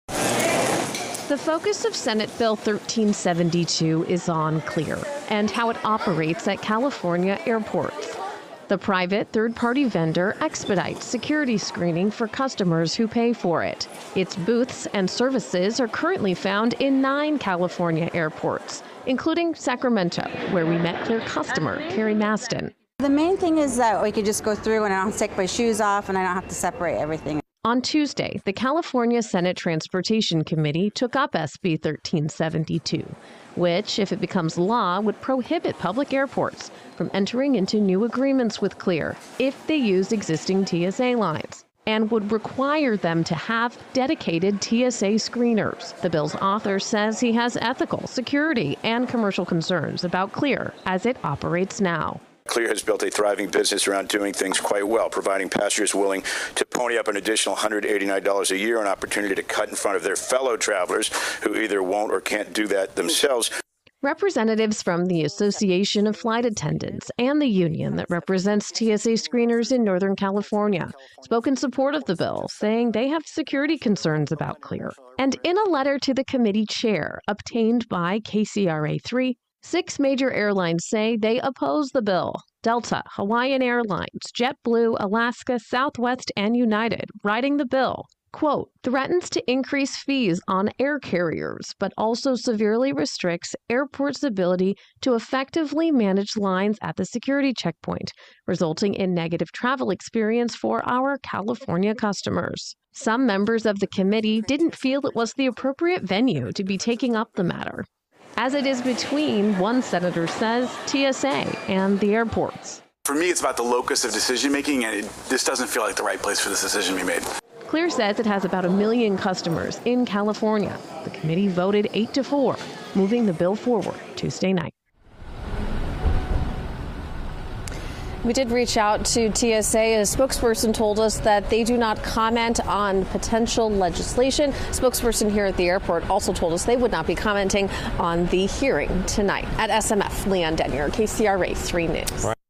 California bill would ban security shortcut at public airports
Senate Bill 1372 would prohibit public airports in California from entering into new agreements with third-party vendors that allow certain passengers to skip ahead of other travelers